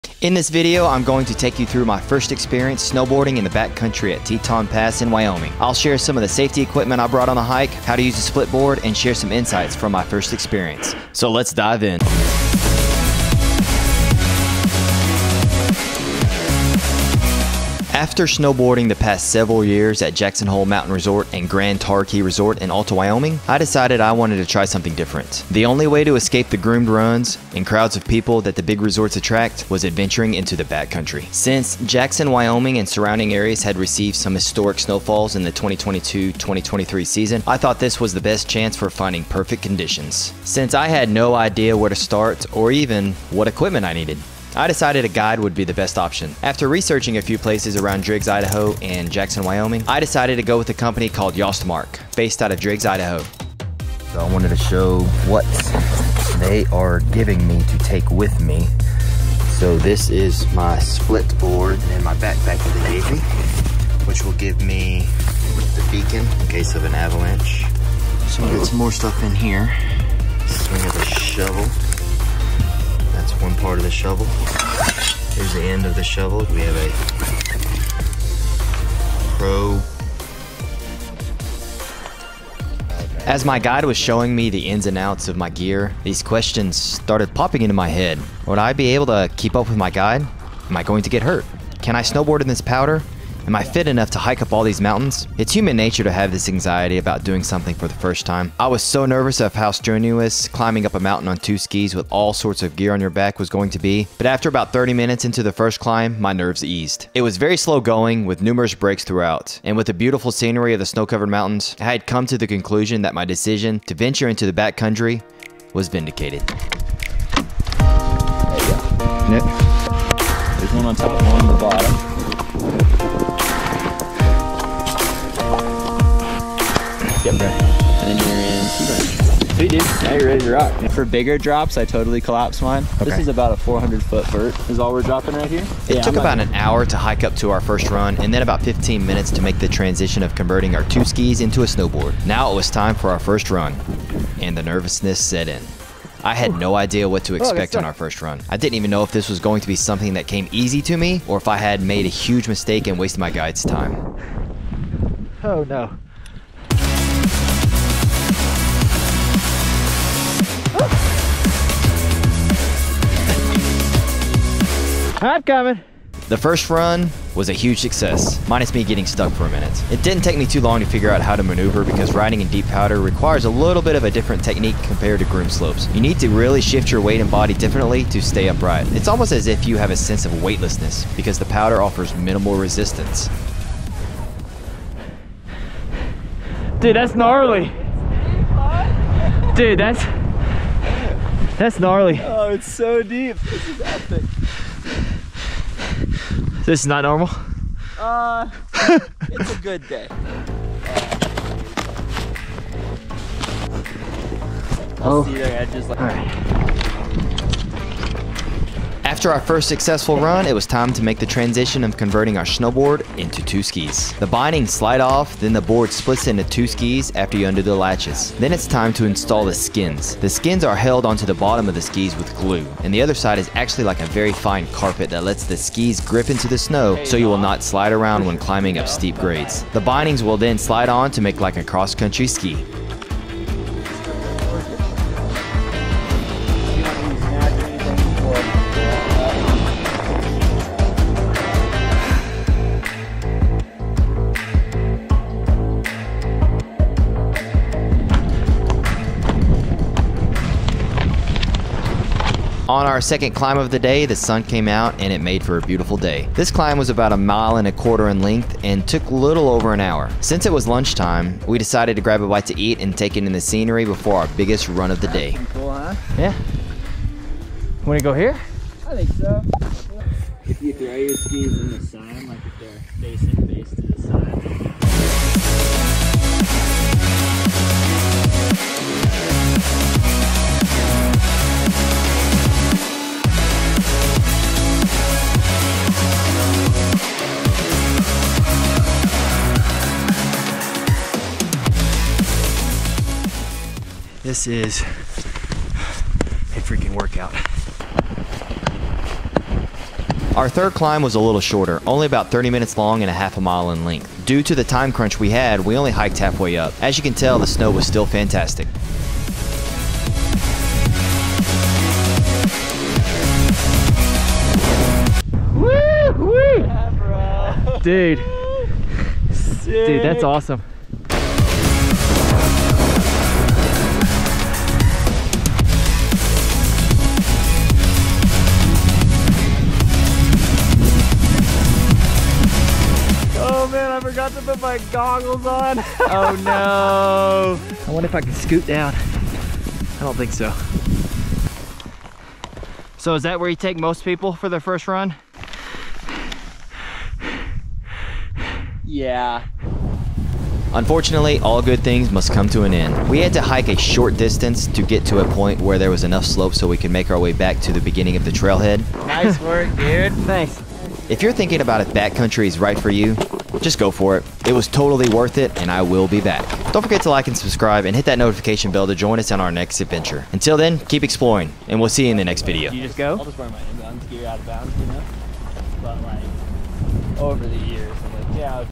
BACKCOUNTRY Snowboarding 101: A Beginner's Guide
In this video, I dive into the world of backcountry snowboarding for the first time at the snowy wilderness of Teton Pass in Wyoming.  This video is perfect for beginners looking to explore the stunning terrain this region has to offer. Join me and my guide as we share essential tips, gear recommendations, safety precautions, and an unforgettable adventure through the Teton Pass.